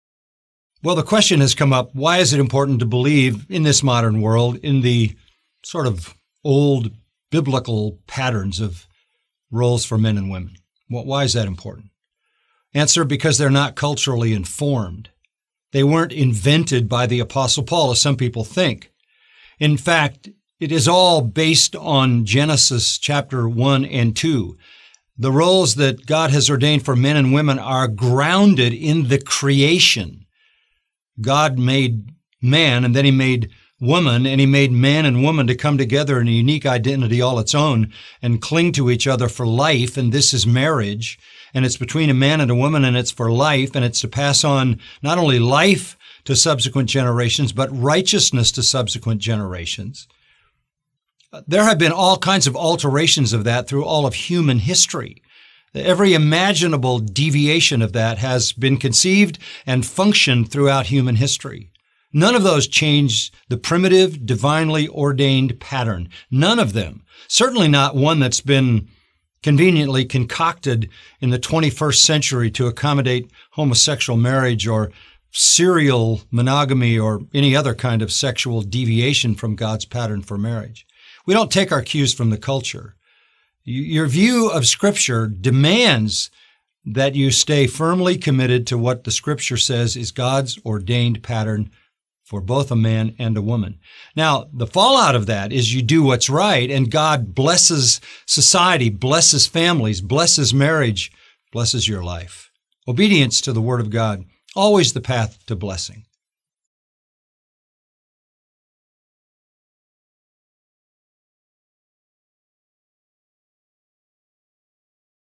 John MacArthur | Rola mužov a žien
Prečo je dôležité veriť starým poriadkom, ktoré určujú úlohy mužov a žien?

Preložené s povolením. Translated by permission.